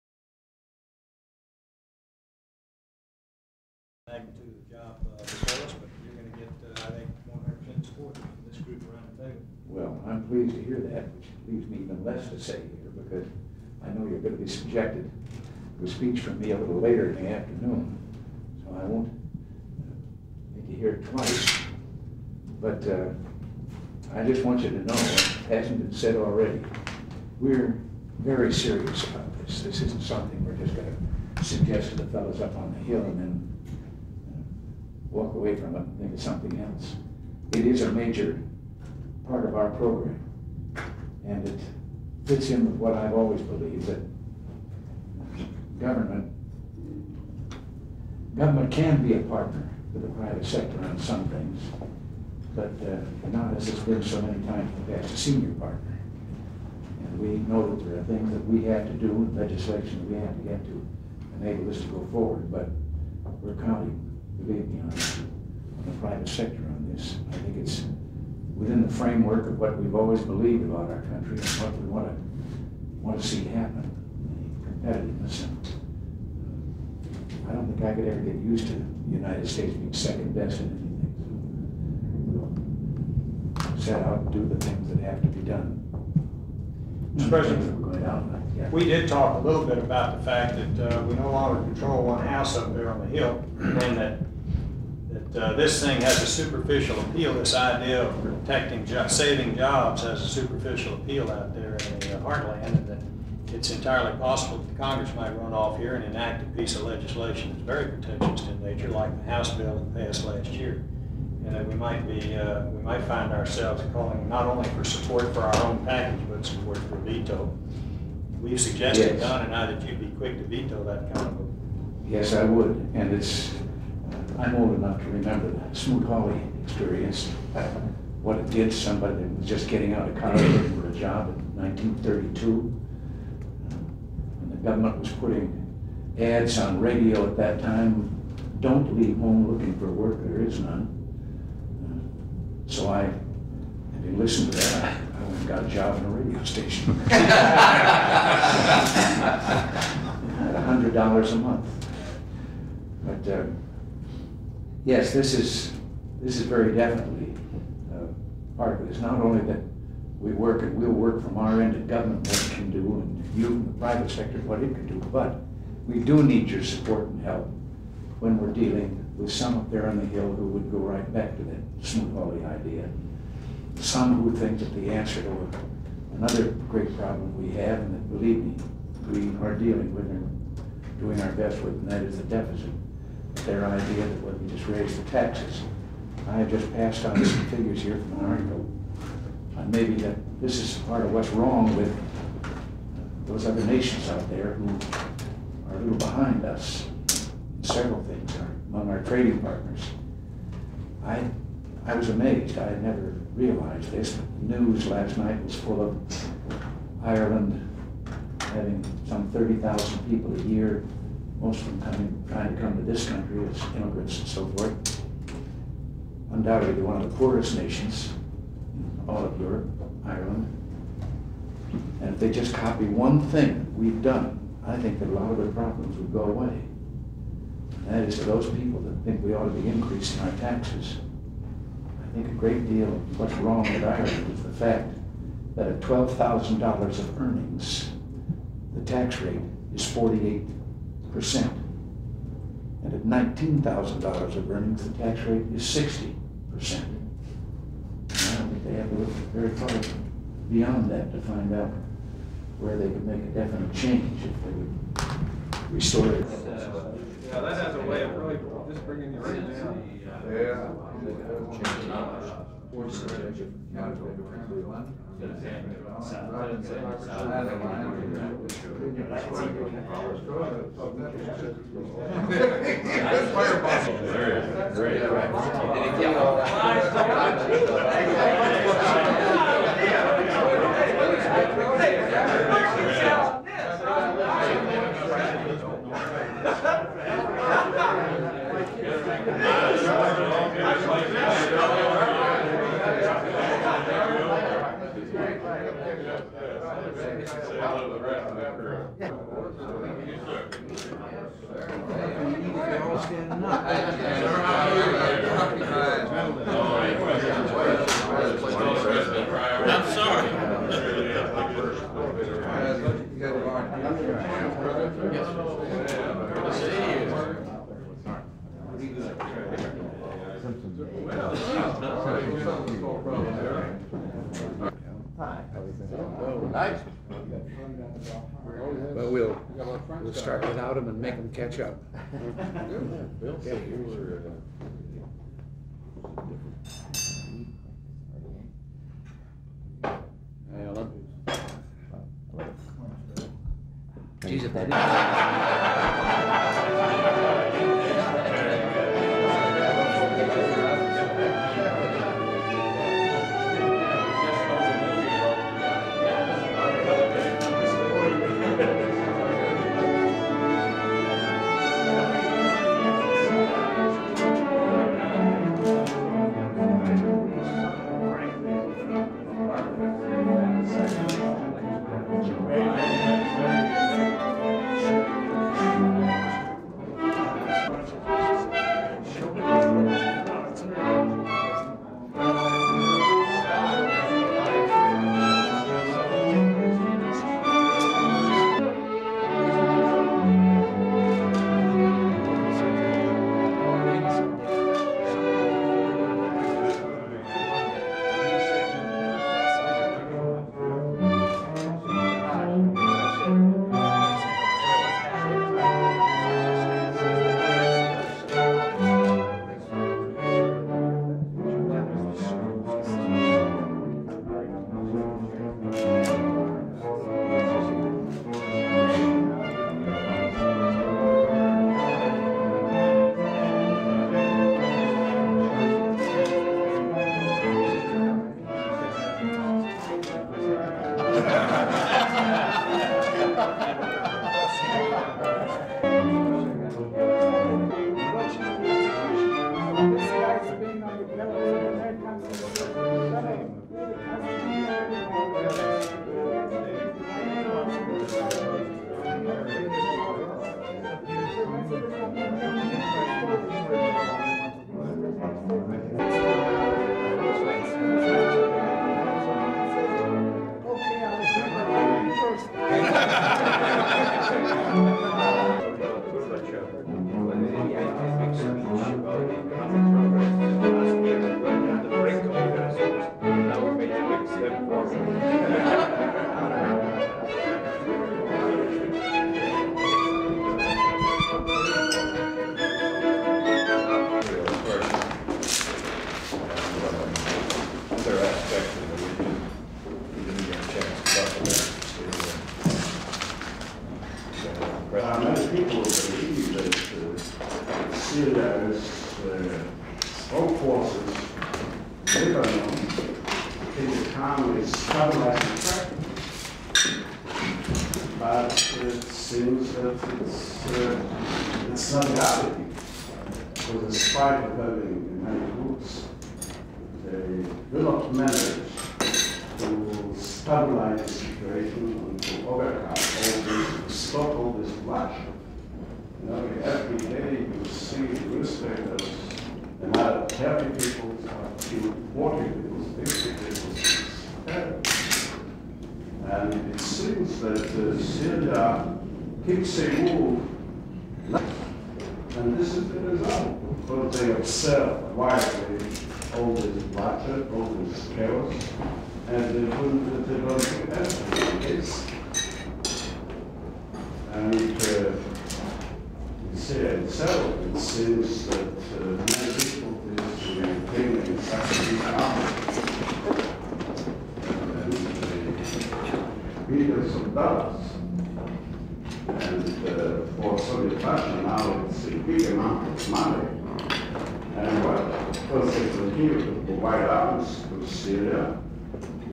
President Reagan's Photo Opportunities on February 17-18, 1987
Full Title: President Reagan's Photo Opportunities on February 17-18, 1987, Drop by Meeting with President Reagan on Competitiveness in Cabinet Room on February 17, 1987, Bipartisan Congressional Breakfast in Cabinet Room, President Reagan having Lunch with Prime Minister Yitzhak Shamir of Israel in Family Dining Room on February 18, 1987

Transcript: N/A

Production Date: 2/17-18/1987

Access Restriction(s):Unrestricted
Use Restriction(s):Unrestricted